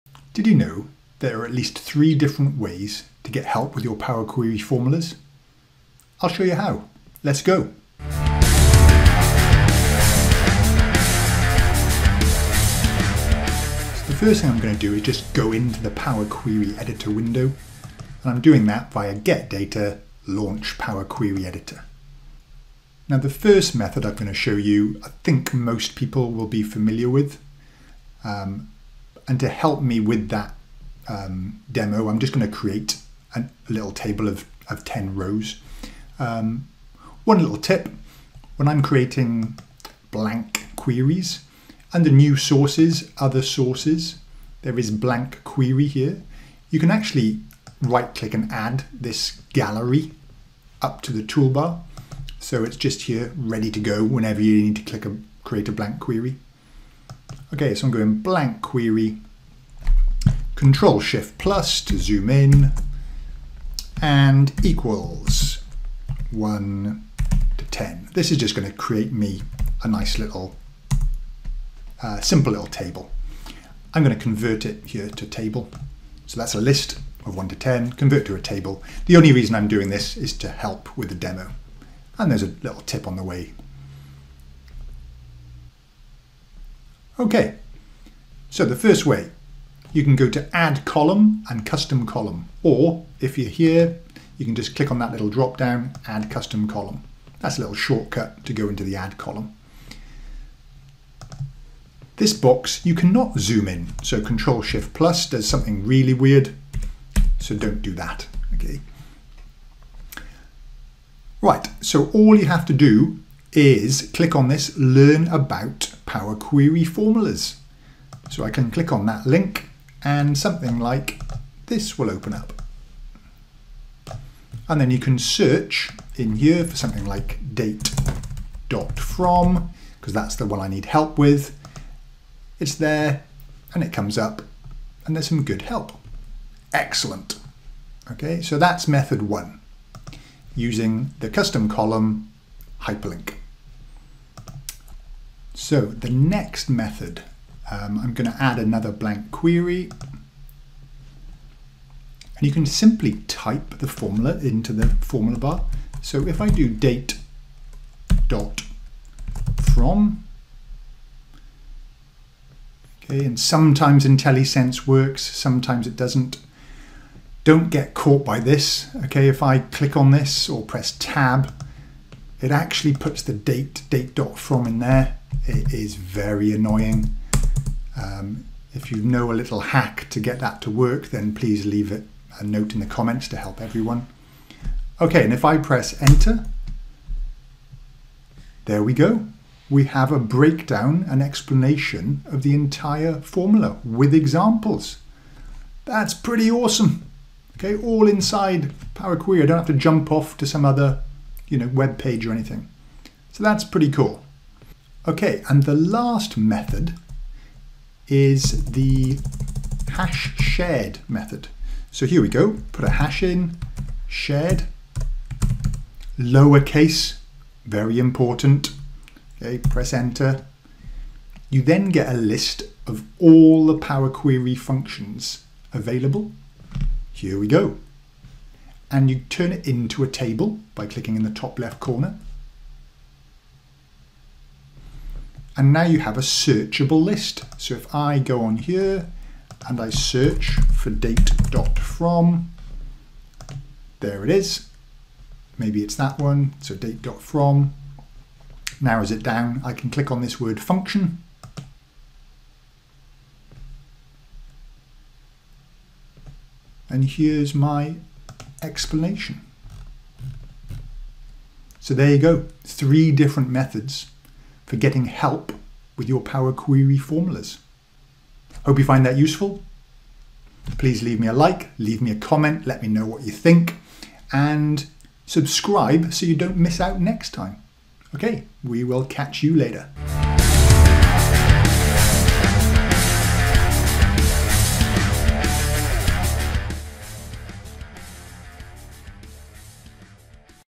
Power Query Formula Help
Need help with your Power Query "M" language functions and formulas?
🔽  Expand to see more info 🔽
Here’s 3 ways to get started:

00:37 Tip on adding Blank Query to Quick Access Toolbar
01:04 Create a small table using {1..10}
01:29  The Custom Column URL link
02:27 The Formula Bar method
03:26